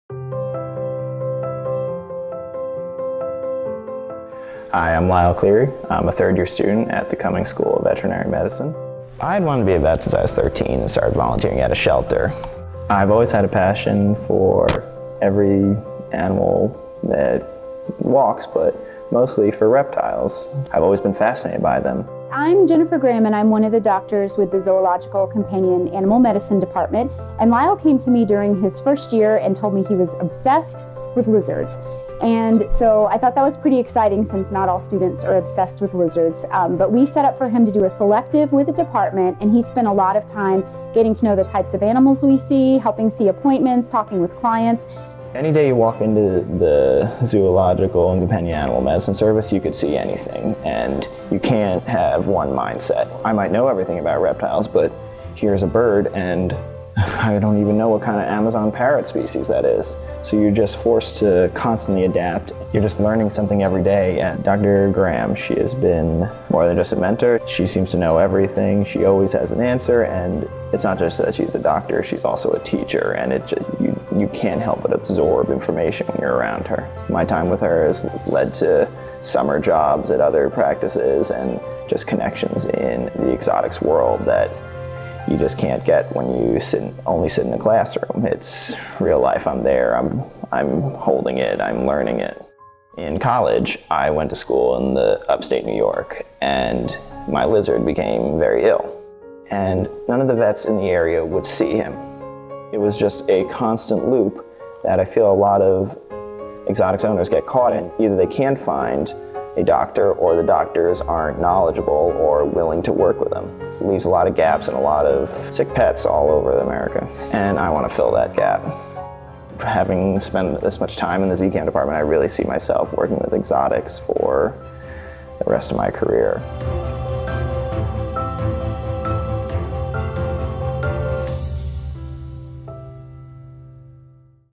Lyle Cleary (V'17), ZCAM Selective
Lyle Cleary, V'17, DVM candidate at Cummings School of Veterinary Medicine as he discusses his Zoological Companion Animal Medicine Selective.